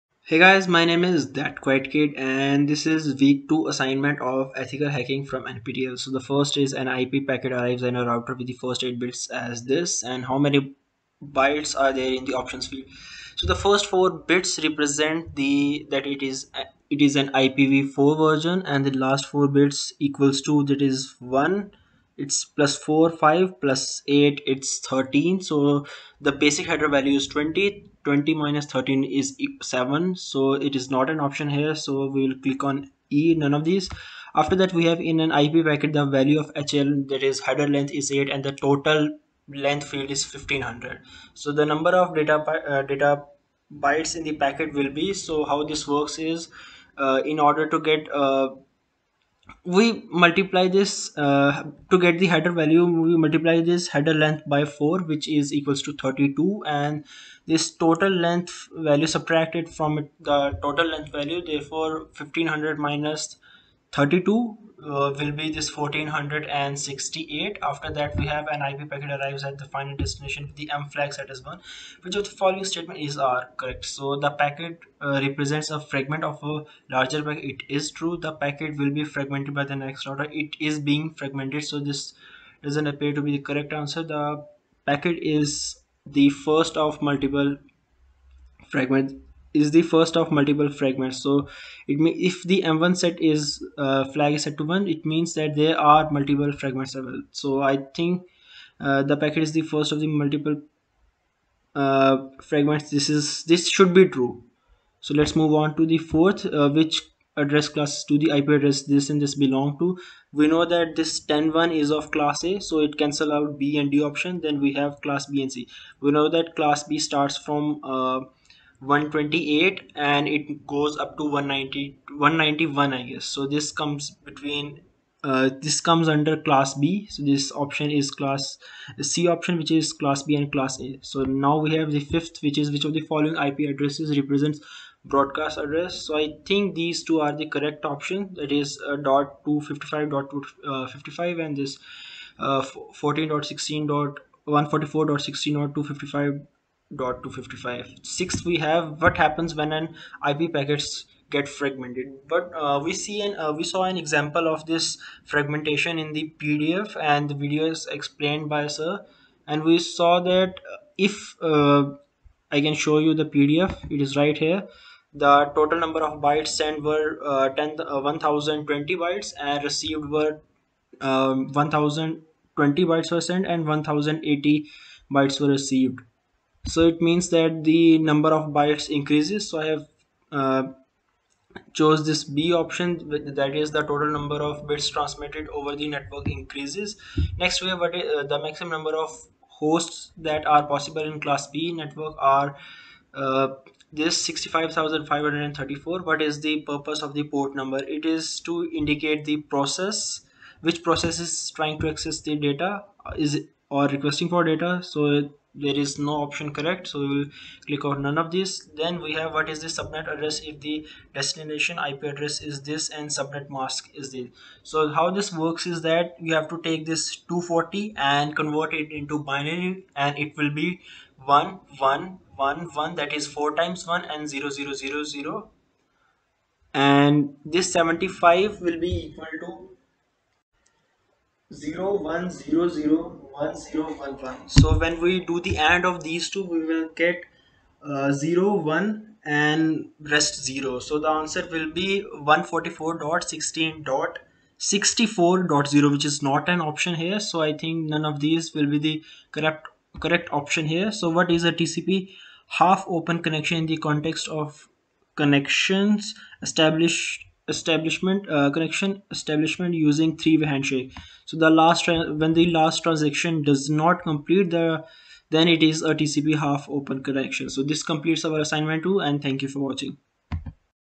NPTEL Ethical Hacking Week 2 Assignment Answers
Answers:-
Q1:-e
Q2:-1468
Q3:-a & c
Q4:-c
Q5:-a & d
Q6:-b
Q7:-65534
Q8:-d
Q9:-d
Q10:-d